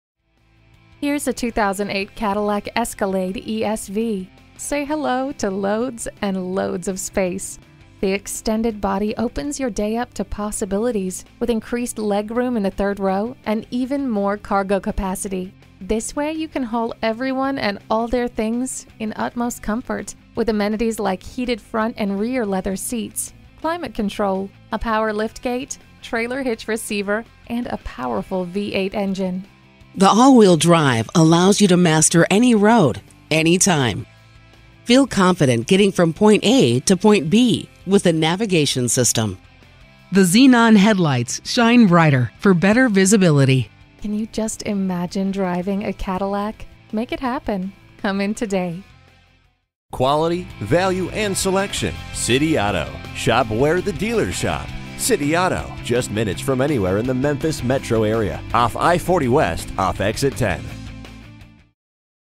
2008 Cadillac Escalade Memphis, TN #329719
Year: 2008
Make: Cadillac
Model: Escalade
Trim: ESV
Engine: 8-Cyl
Transmission: Automatic
Color: White
Interior: Tan
Mileage: 76012
Stock #: 329719
VIN: 1GYFK66828R252905
Certified pre owned. Super nice. Loaded up with Navigation, DVD, Heated Steering Wheel. This pearl with tan combo looks great! Wont last long. Call Chris Weeks for an appointment.

Address:
4932 Elmore Rd